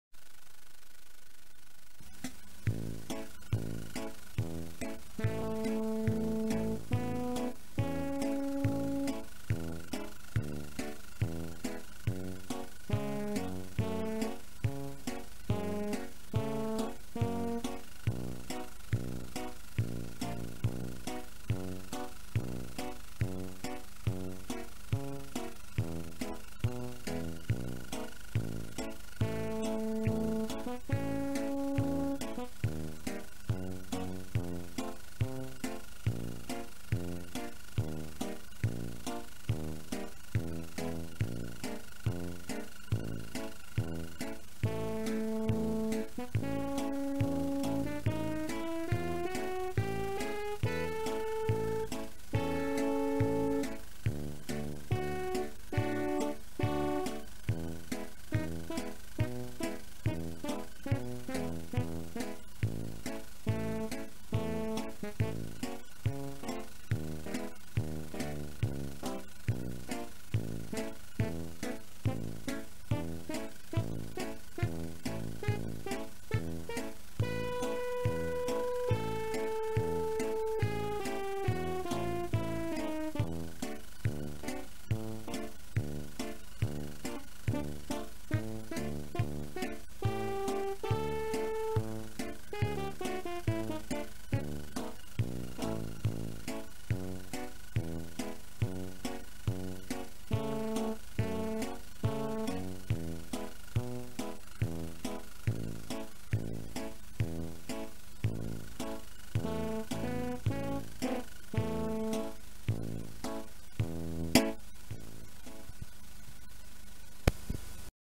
I just like to call it quarantine project 2
Making stuff on the Yamaha PSR-21, recording video and direct audio on a JVC HR-V610AA and digitising on a USB capture card (2)